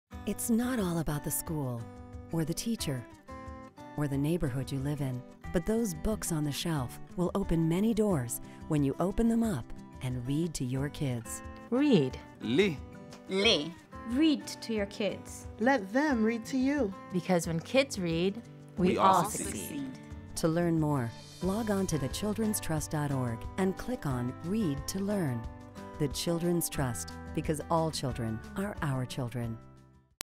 Read to Learn - The Children's Trust :30 Spot
A communitywide initiative with the goal that all children read at or above grade level by the third grade.